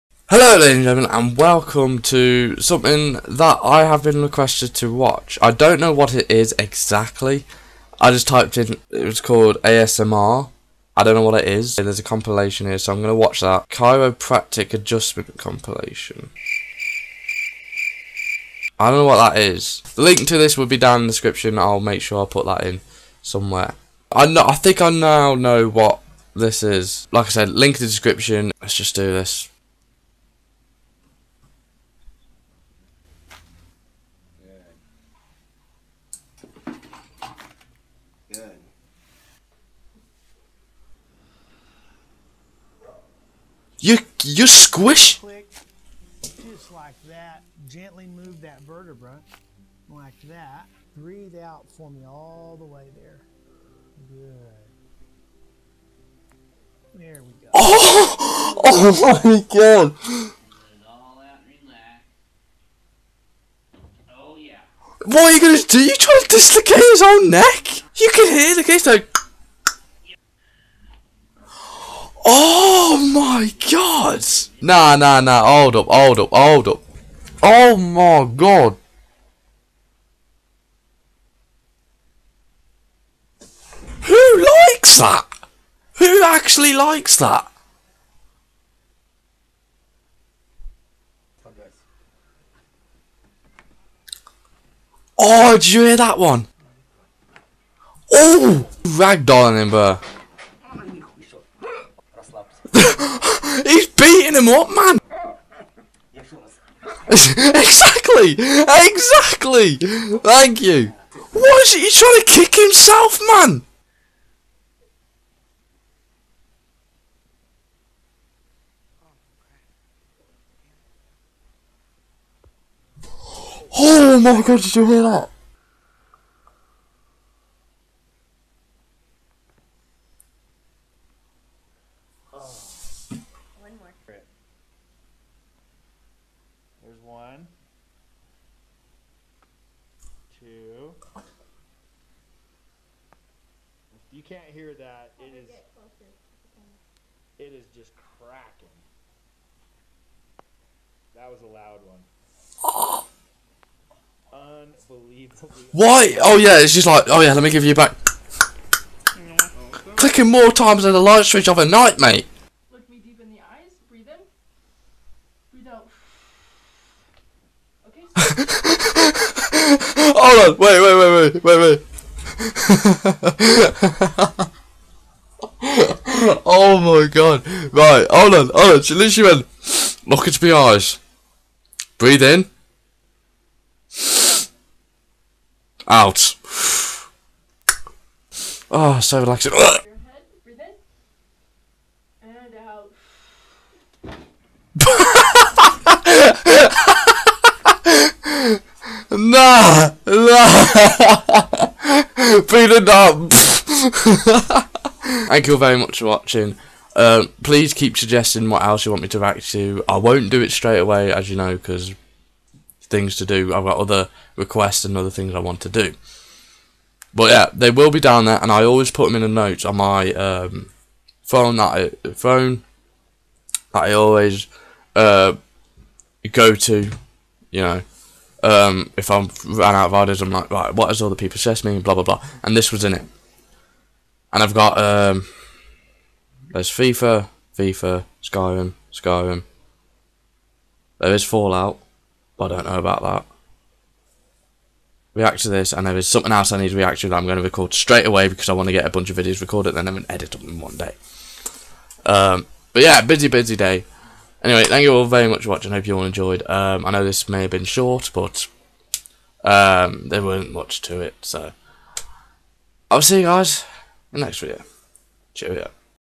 WTF IS THIS!!!!
I react to some crazy asmr compilations... pretty grim.

I am on twitter and on instagram if you want to conatct me to chat or simply follow everything i am doing...not creepy at all.